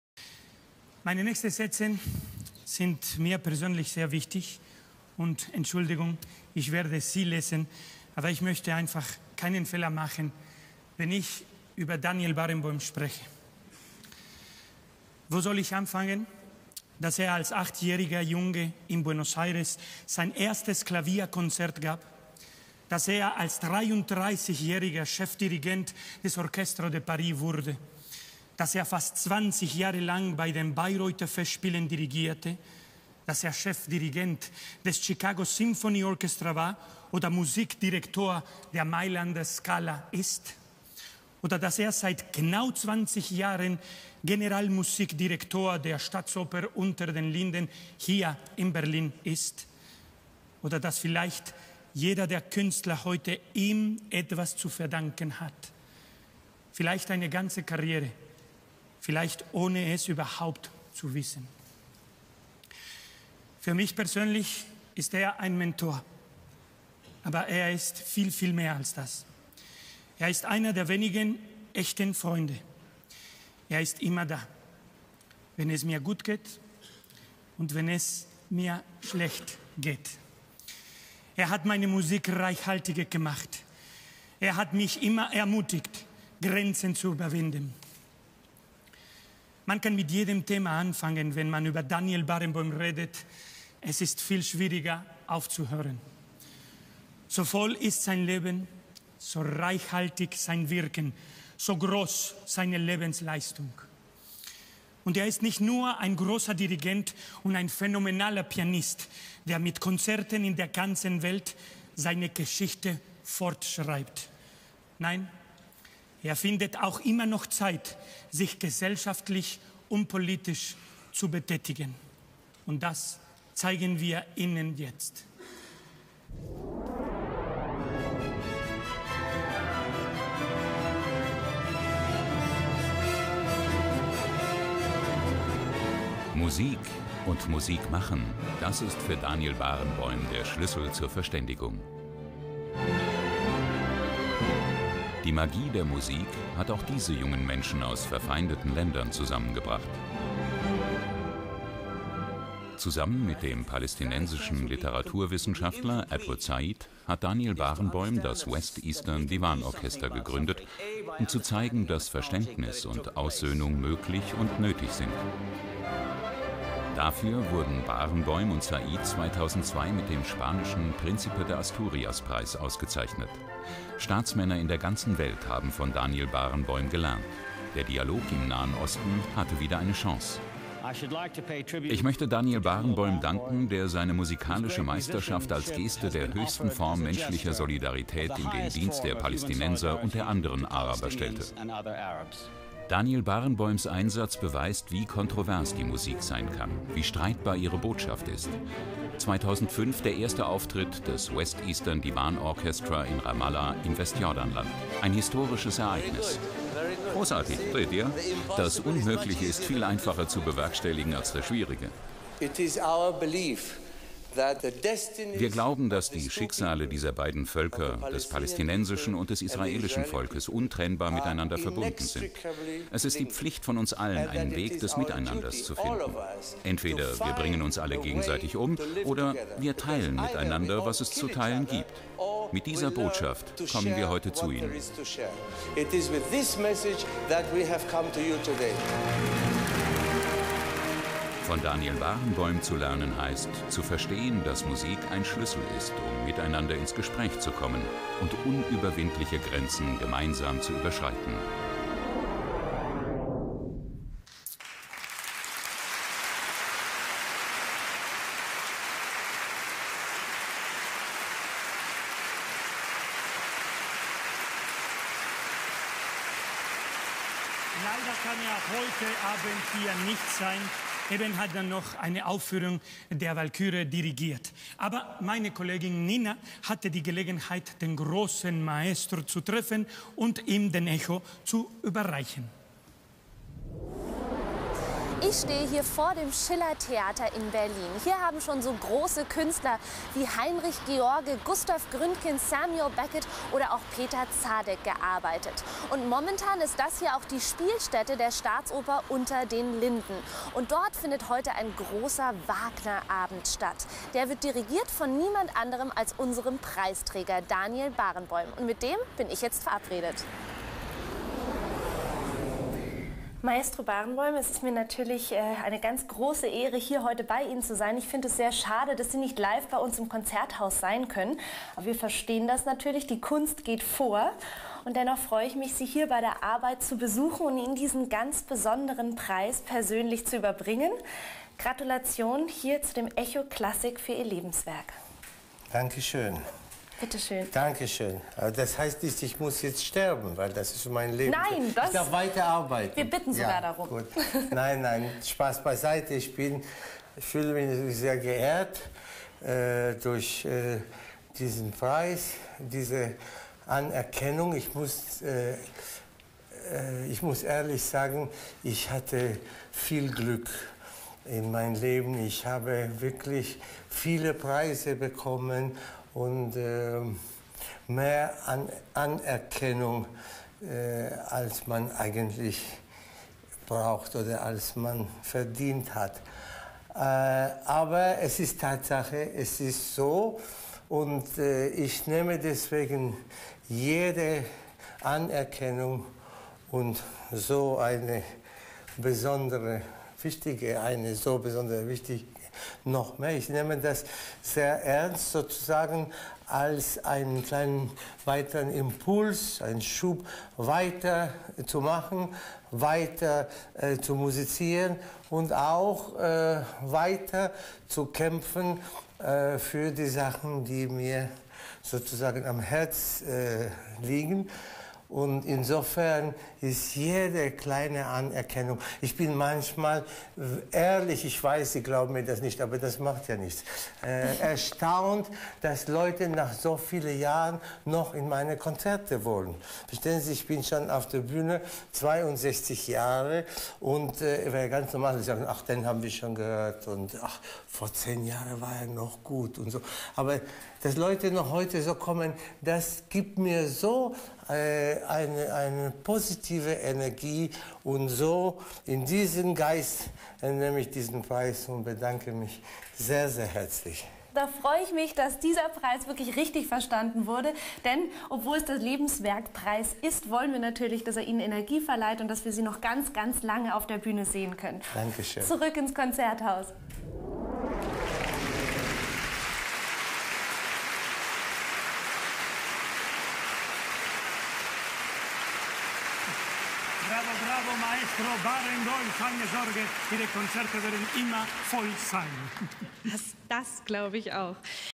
Award for Daniel Barenboim - ECHO KLASSIK 2012 - Rolando Villazón, Nina Eichinger
- Award ·WÜRDIGUNG DES LEBENSWERKES" for Daniel Barenboim - Gala ECHO KLASSIK 2012 - Rolando Villazón, Nina Eichinger
14/10/2012 Konzerthaus Berlin